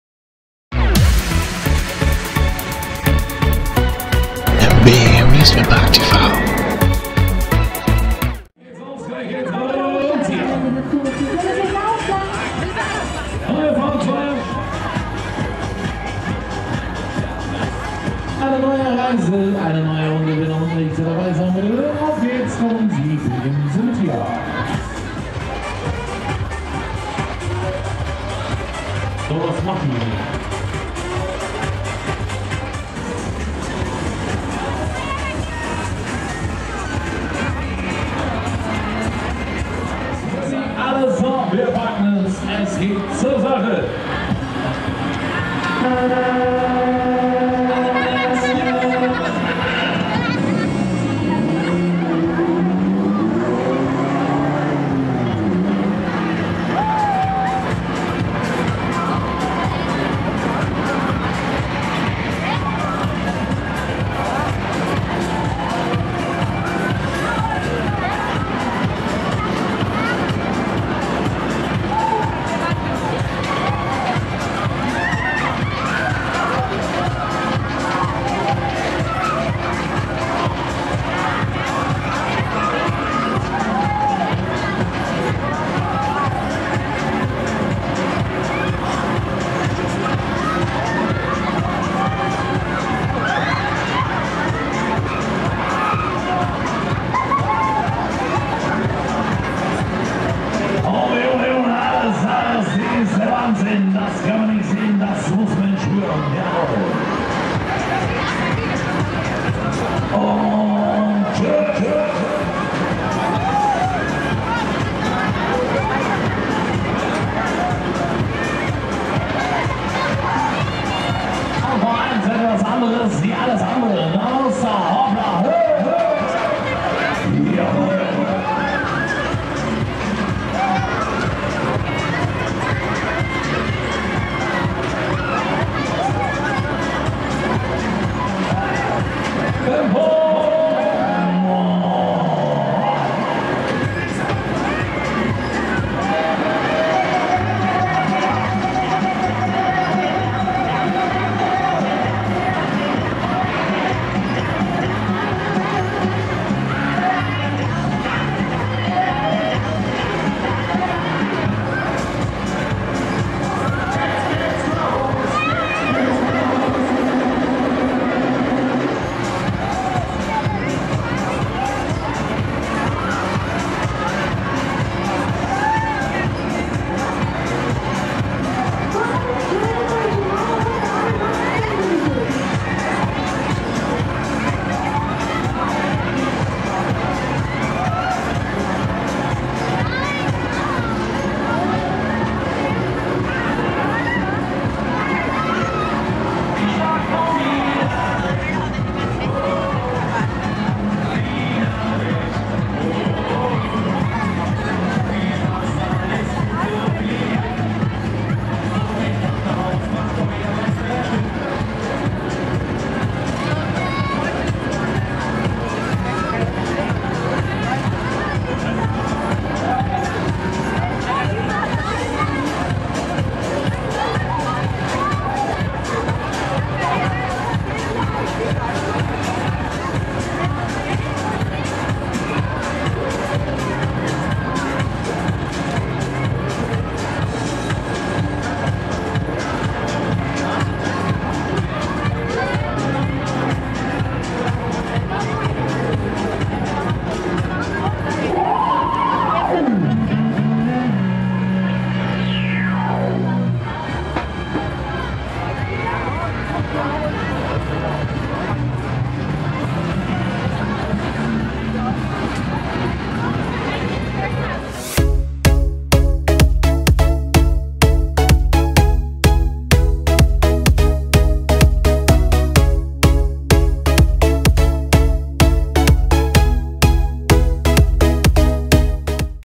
Alpha 1 / Peeters - Herbstsend Münster 2019
Moin!
In diesem Video seht Ihr den Alpha 1, der Schaustellerfamilie Peeters, auf dem Herbstsend 2019 in Münster. Es handelt sich hierbei um den letzten, noch in Deutschland reisenden, Huss UFO. Viel Spaß beim anschauen des Videos!